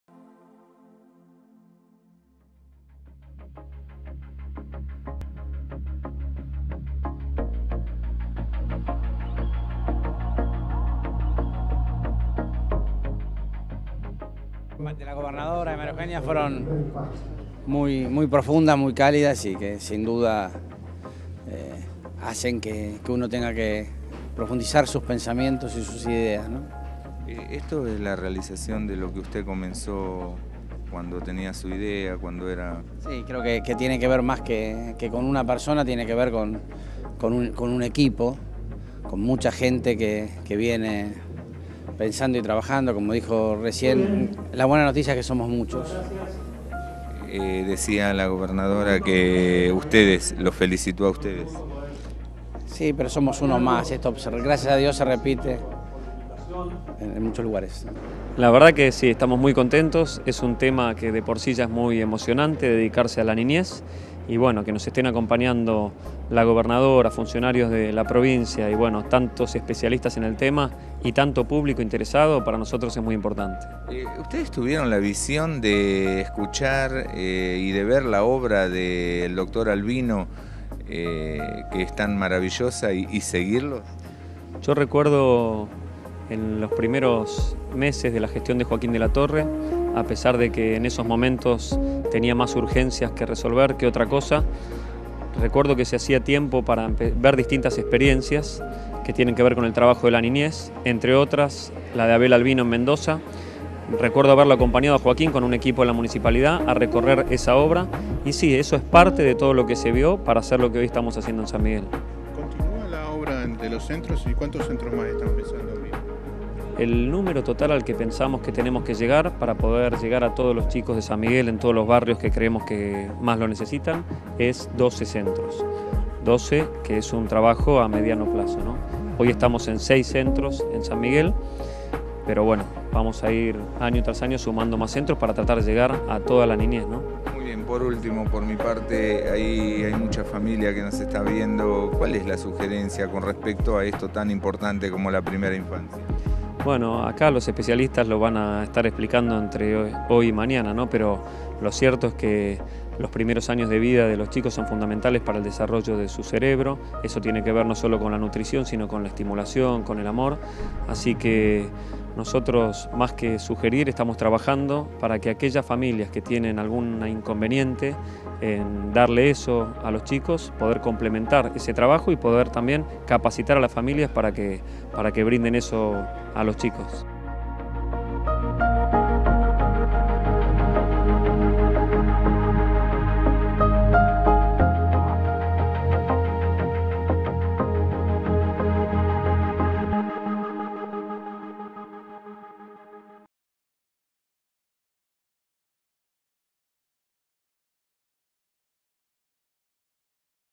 San Miguel - Iº Congreso Internacional de Primera Infancia
I° Congreso Internacional de Primera Infancia, notas con el Ministro de la Producción bonaerense Dr. Joaquín De la Torre e Int. Jaime Mendez.